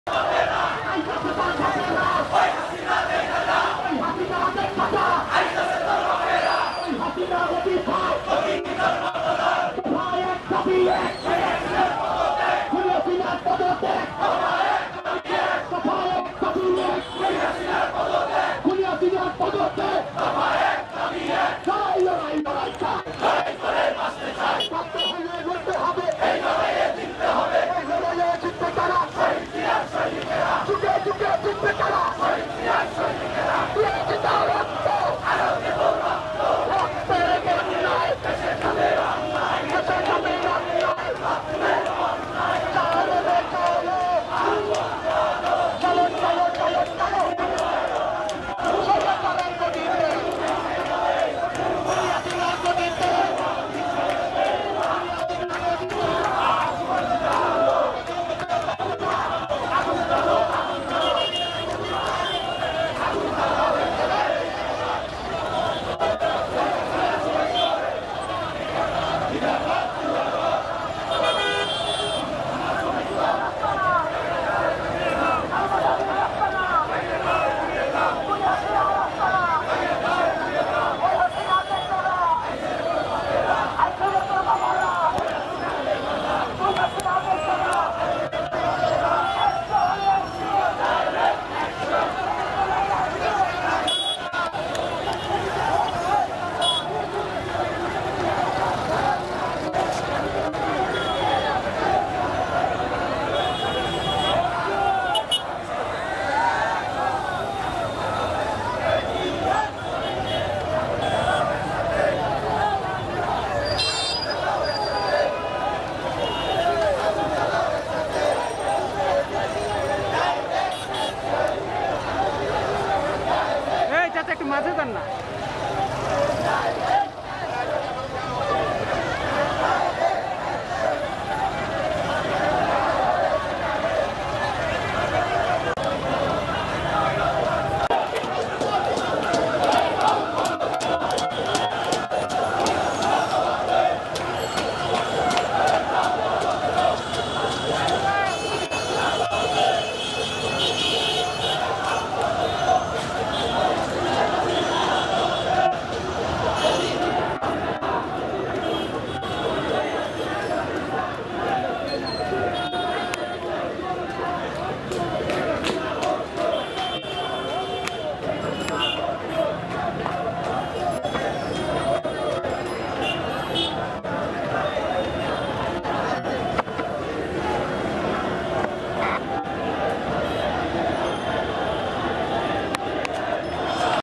আর কোন ছাড় নয় !! এই মুহুর্তে গদি ছা*ড় ! স্লোগানে কাঁপছে ঢাকার রাজপথ ! দেখুন
ফুঁসে উঠলো প্রিয় নেত্রীর জন্য ! এই মুহুর্তে গদি ছাড় ! স্লোগানে কাঁপছে ঢাকার রাজপথ ! দেখুন